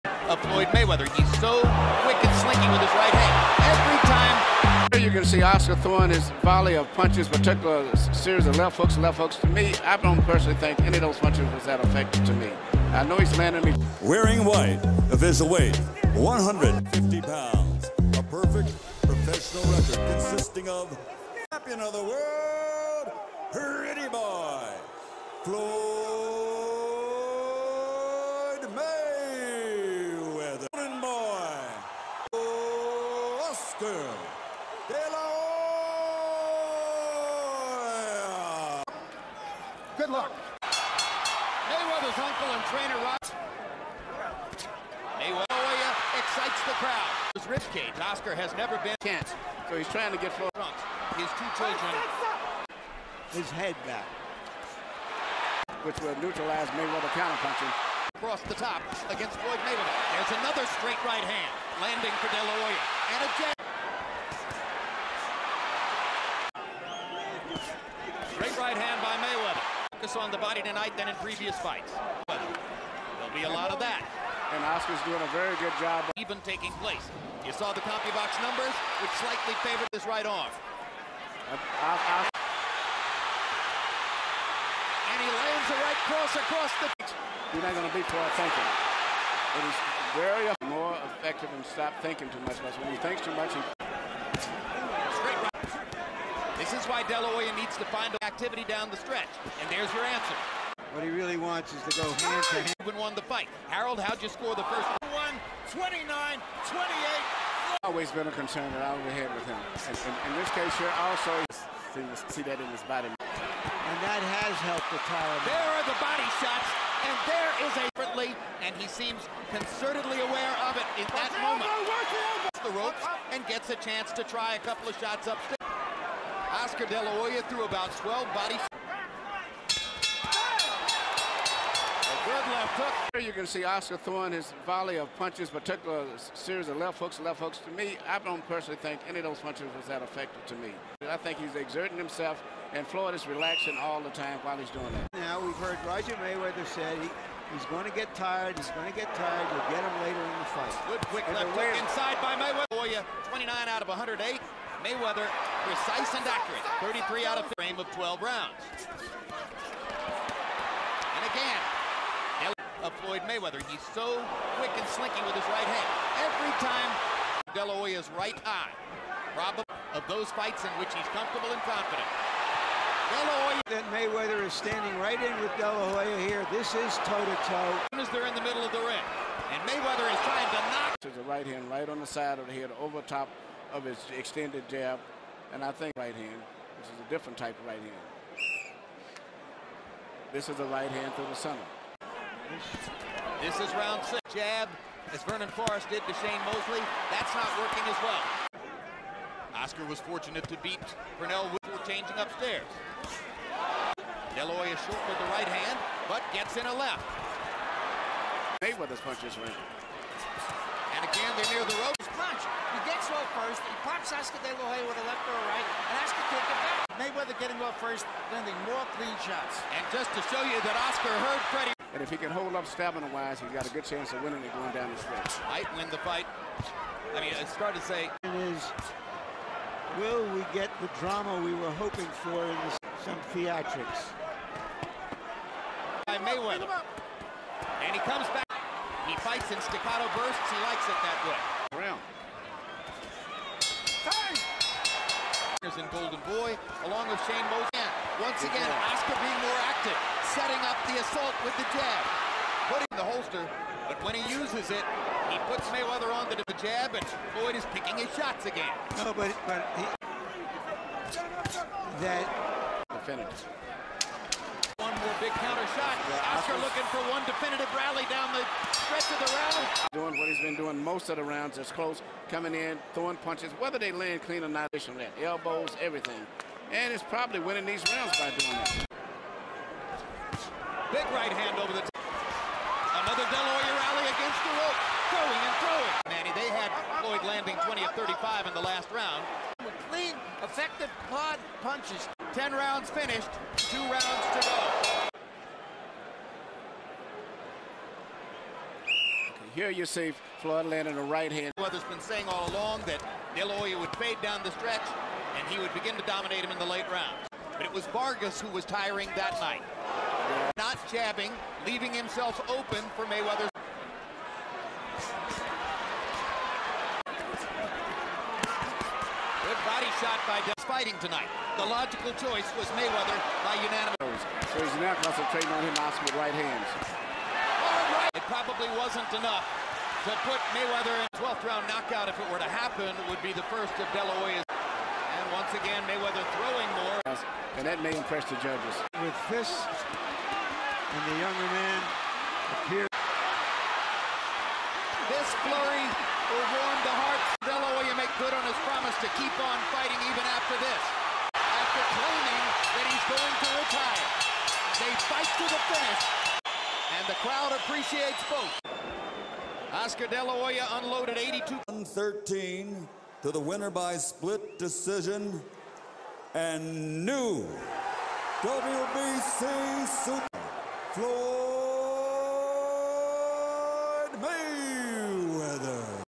Floyd Mayweather Jr contra 0scar De La H0ya highlights
Floyd Mayweather Jr.contra 0scar De La Hoya  billed as The World Awaits, was a super welterweight superfight that took place  05 May 2007